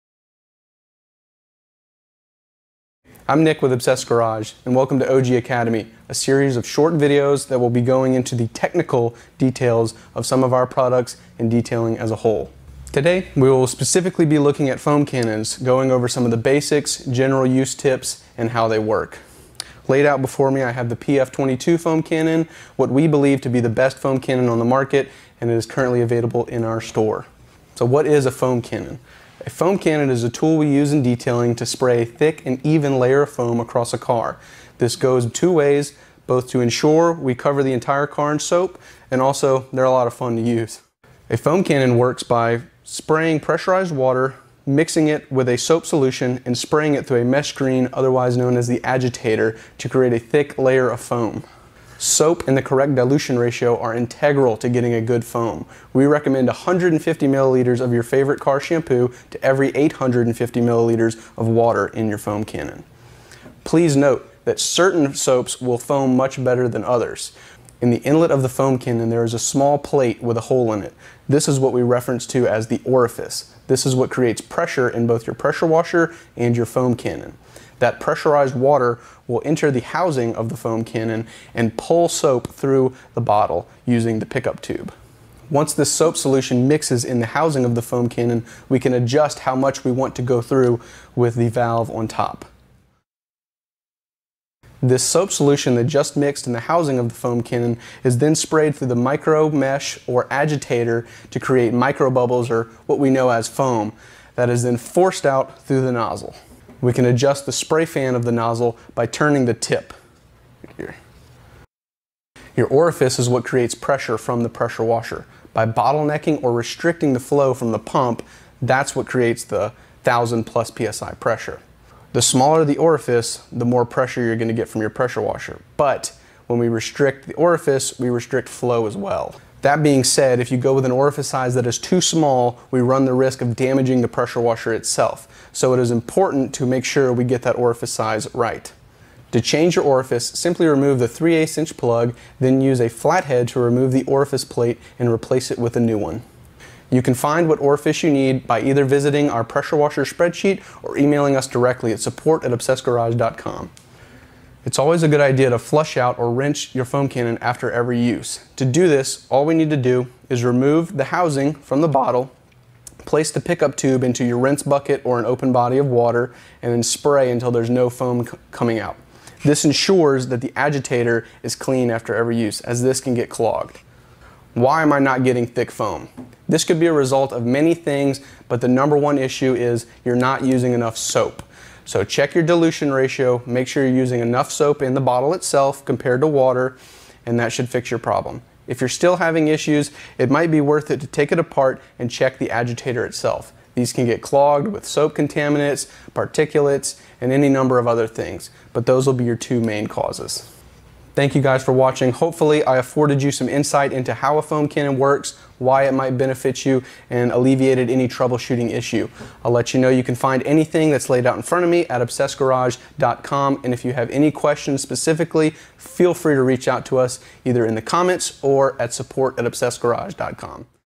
Foam Cannon 101: Foam Cannon Maintenance And Usage With The PF22.2
MTM PF22.2 Foam Cannon

What is the Best Foam Cannon?

For more insight into my life and business become a member of Inside the Hex

Gear We Use Everyday (Cameras, Computers, Etc.):

Timecodes
0:00 - Intro
0:28 - What is a foam cannon?
0:40 - How a foam cannon works
1:07 - Parts and Operation
2:19 - Changing the Orifice
2:33 - Good Care
2:54 - Troubleshooting
3:21 - Outro